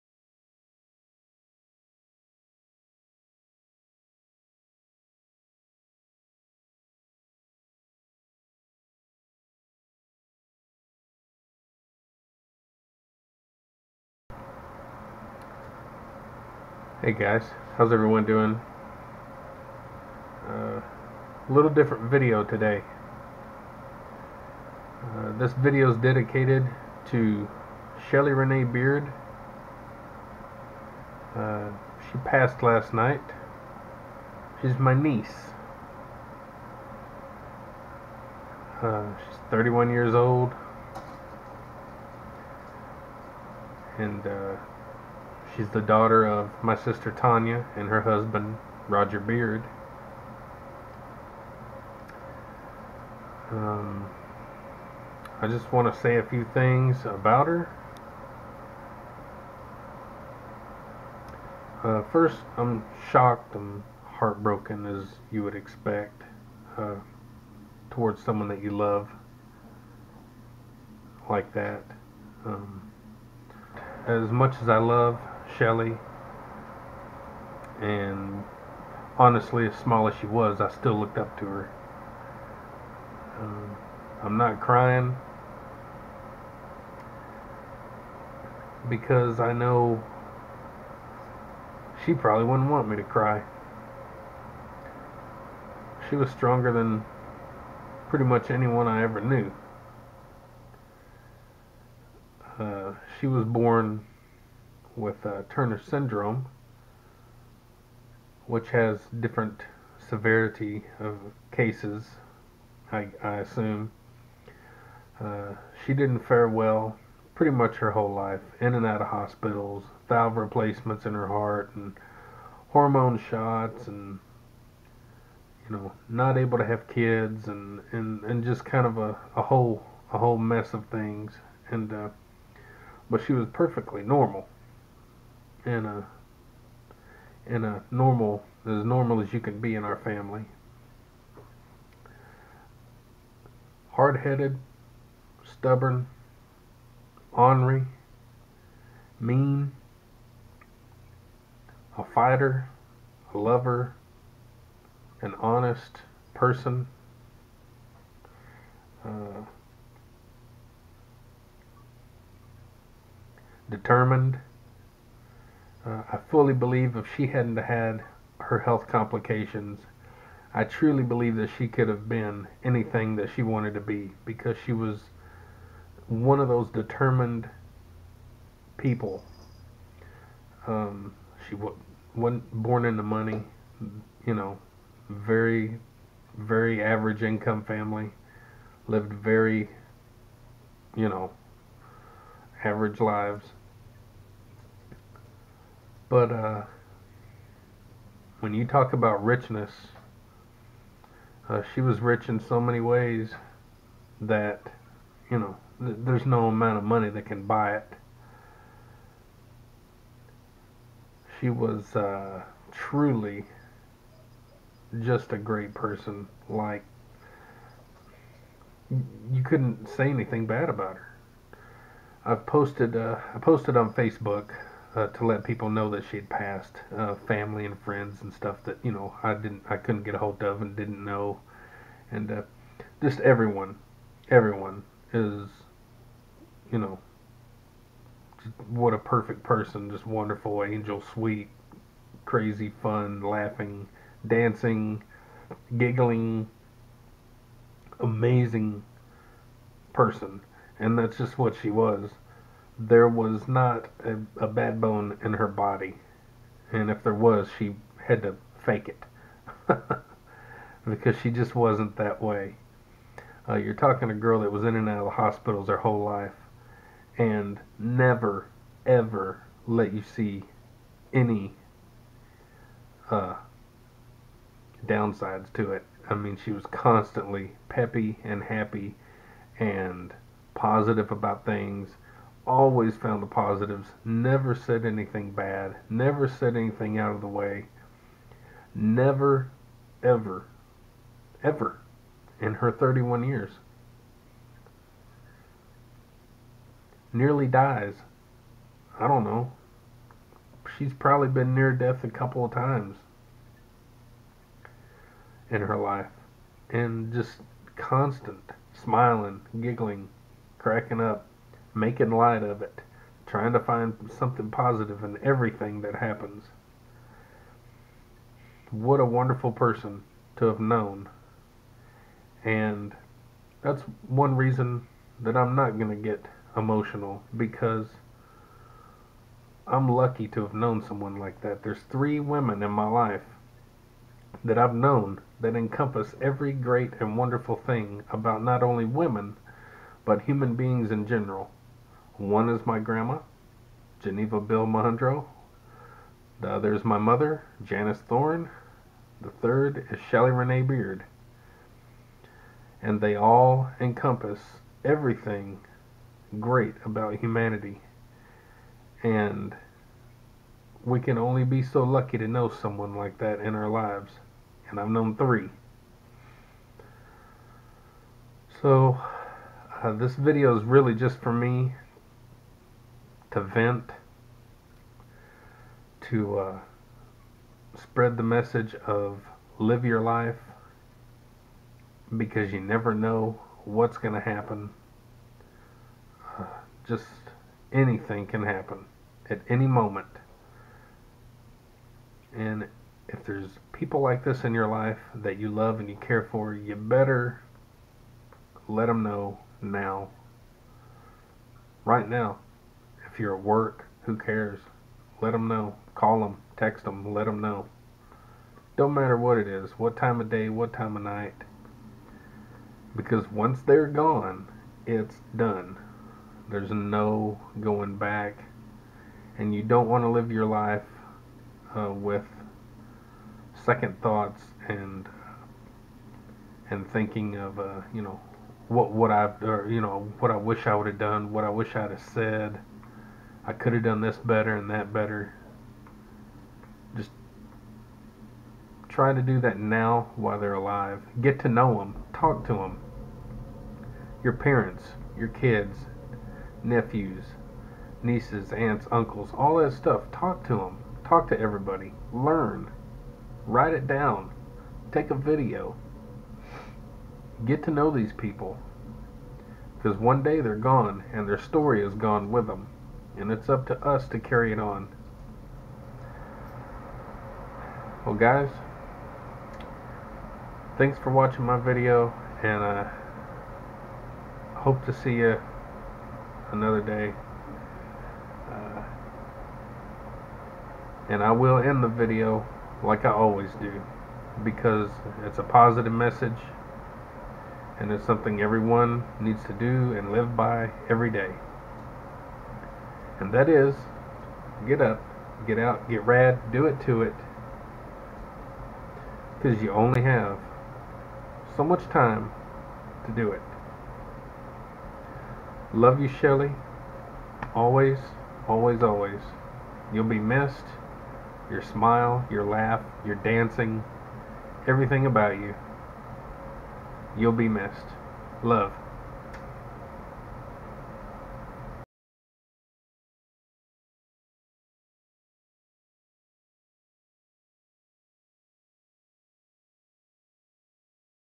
The Death of My Special One of a Kind Niece Shelly 12 17 19
My wonderful loving awesome incredible funny goofy Niece Shelly passed 12-16-19 and I have a few words to say. What a special lil lady..love always Uncle Mike...